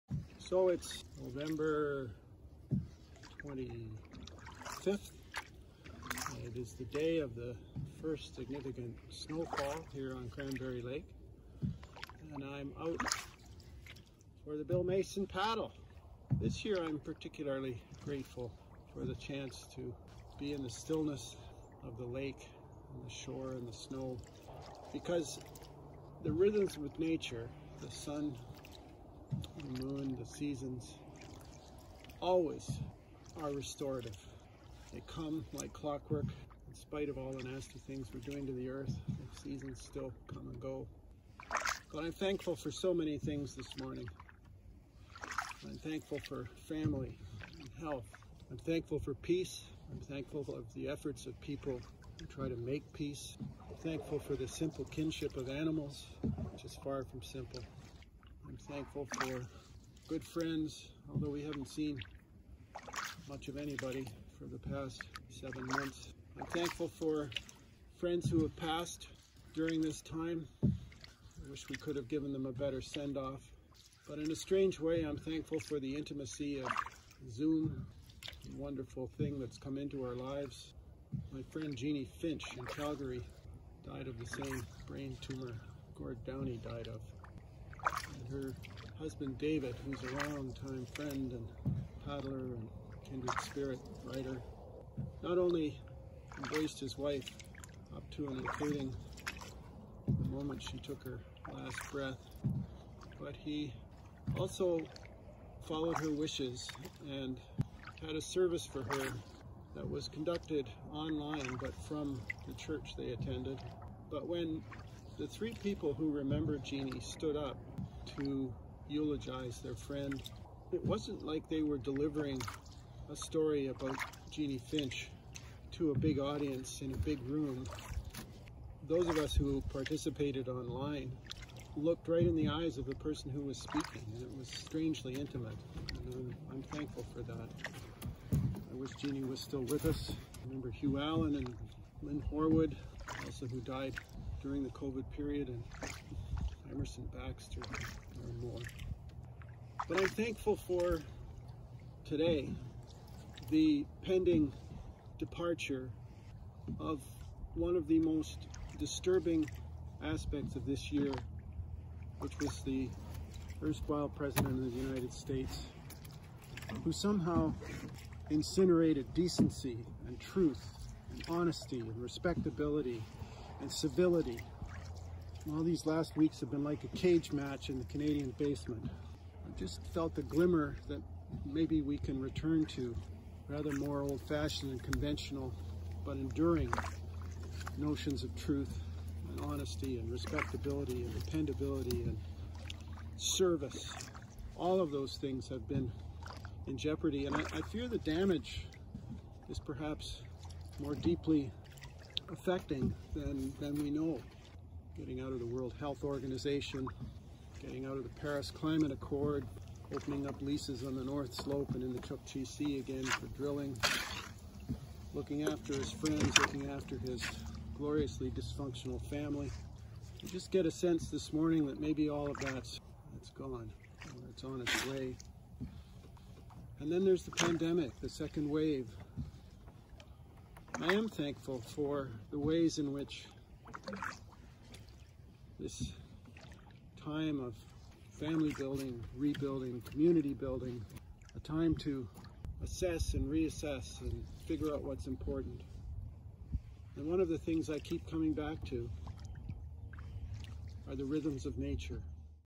Bill Mason Paddle 2020
Each year on the day of the first significant snowfall there are those of in the boreal canoeing community around the world who put our boats in the water and remember Bill Mason and a snowy scene from his final film, Waterwalker.  For me, it's a time to count blessings, to reflect and to soak up all there is to be thankful for.  This year, having learned some rudimentary video production skills thanks to the pandemic, I documented what was a glorious morning on Cranberry Lake in Eastern Ontario and present a snippet or two to encourage others to do the same in your corner of the world, before you put your boats away for winter.